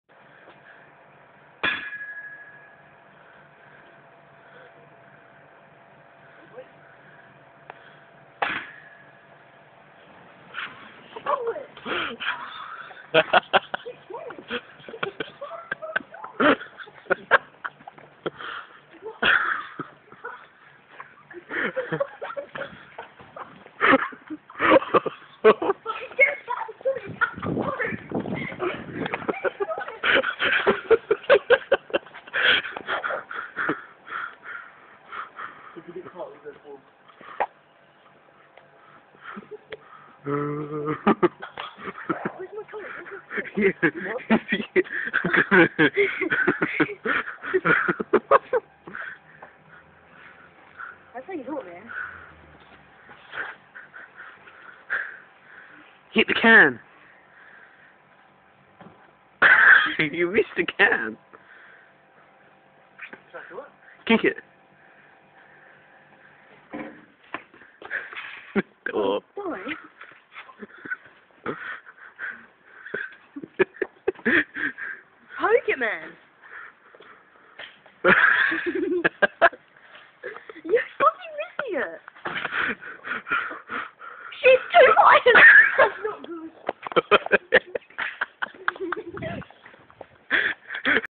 Canola spray on fire
bang the top off a canola can and light it FUNNY!